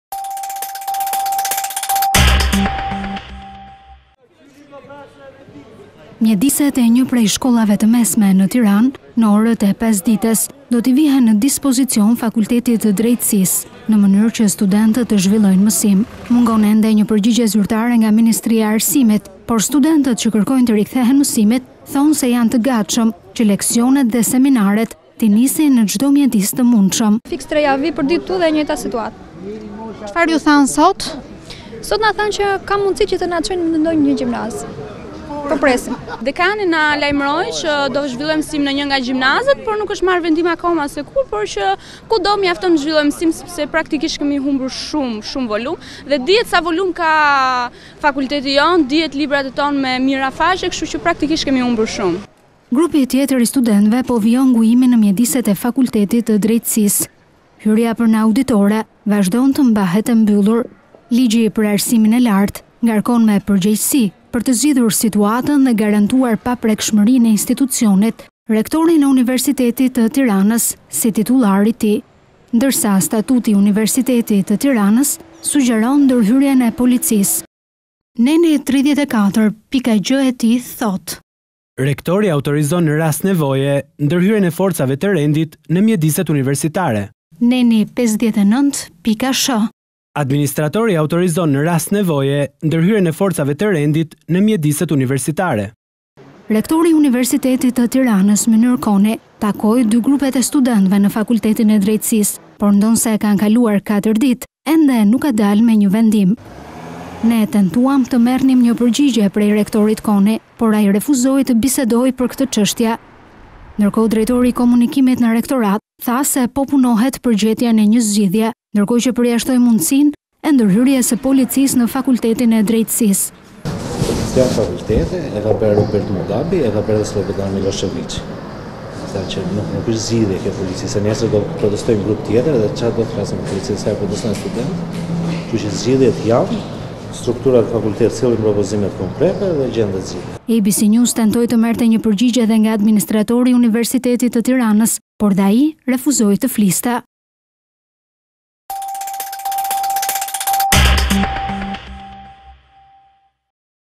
Bllokimi i fakultetit, Juridiku mesim në gjimnazet e Tiranes | ABC News Albania
Mjediset e një prej shkollave të mesme në Tiranë në orët e pasdites do t’i vihen në dispozicon Fakultetit të Drejtësisë në mënyrë që studentët të zhvillojnë mësim.  “Ismail Qemali” dhe “Eqerem Çabej” janë dy gjimnazet që përfliten ndërkohë që përjashtohet “Petro Nini Luarasi” për të shkak të afërsisë me mjediset e fakultetit ku qëndron i ngujuar grupi tjetër i studentëve që kërkojnë anulimin e ligjit për Arsimin e Lartë. Mungon ende një përgjigje zyrtare nga Ministria e Arsimit, por studentët që kërkojnë t’i rikthehen mësimit thonë se janë të gatshëm që leksionet dhe seminaret t’i nisin në çdo mjedis të mundshëm.

Grupi tjetër i studentëve po vijon ngujimin në mjediset e fakultetit të Drejtësisë. Hyrja për në auditore vazhdon të mbahet e mbyllur. Ligji për Arsimin e Lartë ngarkon me përgjegjësi Rektorin e Universitetit të Tiranës, si titullar I tij, për të zgjidhur situatën dhe  garantuar paprekshmërinë e institucionit. Statuti i Universiteti të Tiranës sugjeron ndërhyrjen e policisë.

Neni 34 pika gje e tij thotë; "Rektori autorizon në rast nevoje ndërhyrjen e forcave të rendit në mjediset universitare".

Neni 39 pika sh;"Administratori autorizon në rast nevoje ndërhyrjen e forcave të rendit në mjediset universitare".

Rektori i Universitetit të Tiranës Mynyr Koni takoi dy grupet e studentëve në Fakultetin e Drejtësisë, por ndonëse kanë kaluar katër ditë ende nuk ka dalë me një vendim. Ne tentuam të merrnim një përgjigje prej Rektorit Koni, por ai refuzoi të bisedojë për këtë çështje.

Ndërkohë drejtori i Komunikimit në Rektorat tha se po punohet për gjetjen e një zgjidhje ndërkohë që përjashtoi mundësinë e ndërhyrjes së policisë në Fakultetin e Drejtësisë. "Abc news" tentoi të merrte një përgjigje dhe nga Administratori i Universitetit të Tiranës, por dhe ai refuzoi të fliste./abcnews.al